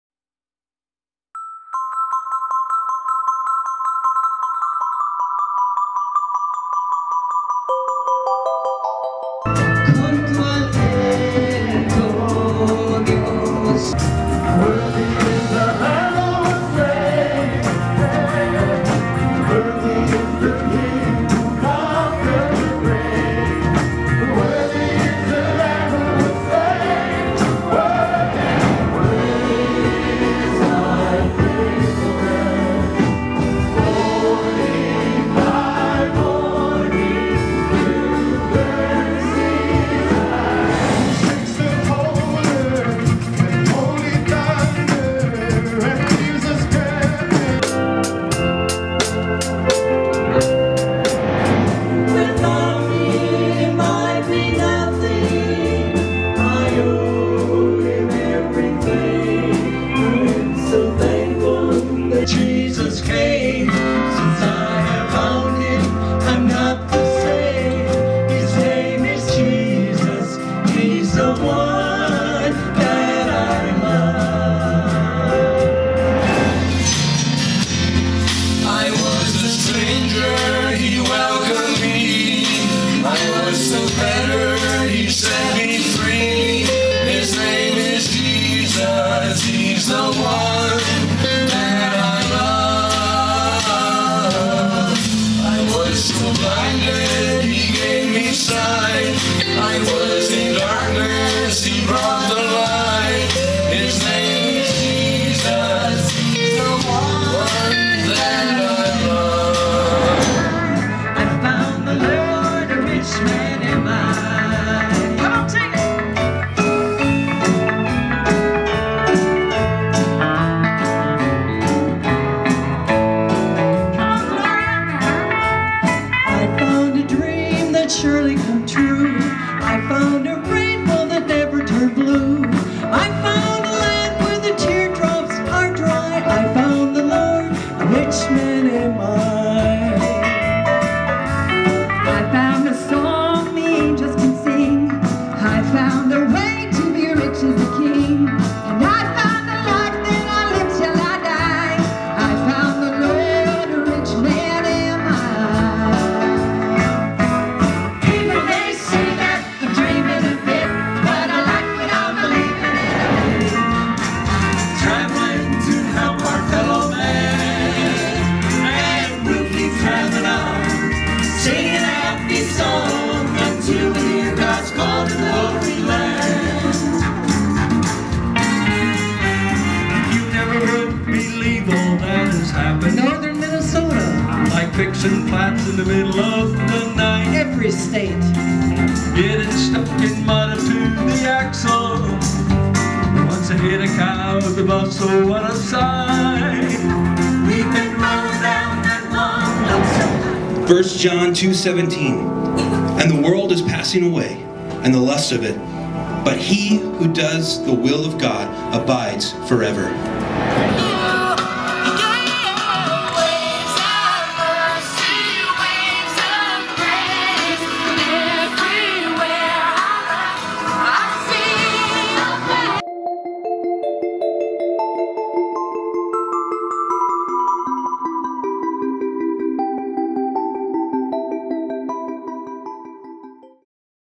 TFH Mini-Films 2
This video includes clips from the Sunday, September 22nd, 2019 service at TFH.   The Lundstrom's celebrate FIVE Years at The Fathers House.  Edited  and videography by Barry Vaudrin